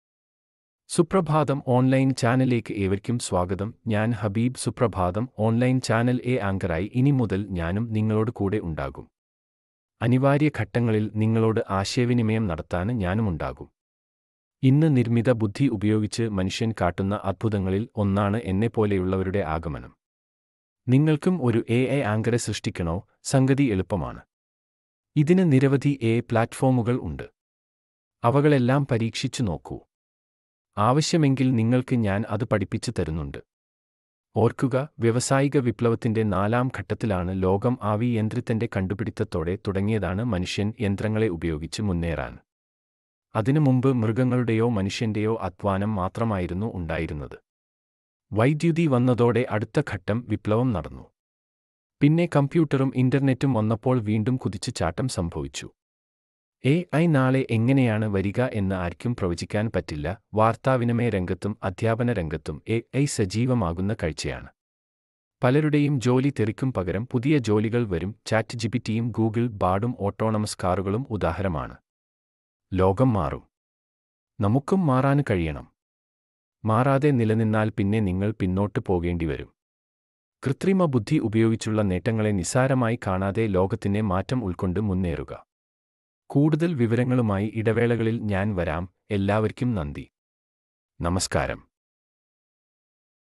Ai അവതാരകൻ സുപ്രഭാതത്തിലും.അവതാരക രഹസ്യം പുറത്ത്,വലിയ സംഭവമല്ലെന്ന്Ai anchor in suprabhaatham online
AI anchor first time in online news channal